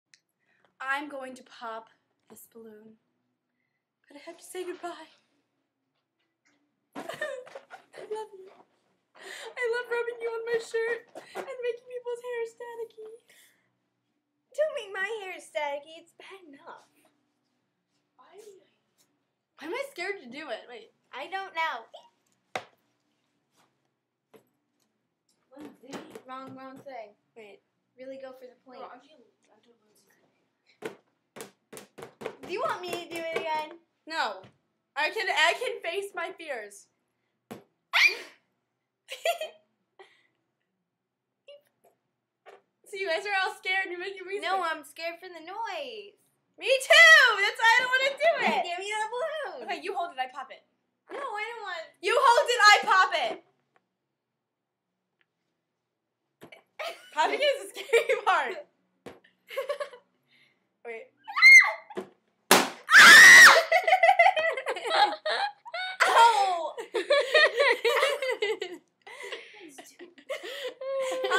Afraid to Pop a Balloon- FLiPAROO990
The story of a girl who is afraid to pop a balloon. Very sad.
:D
FLiPAROO990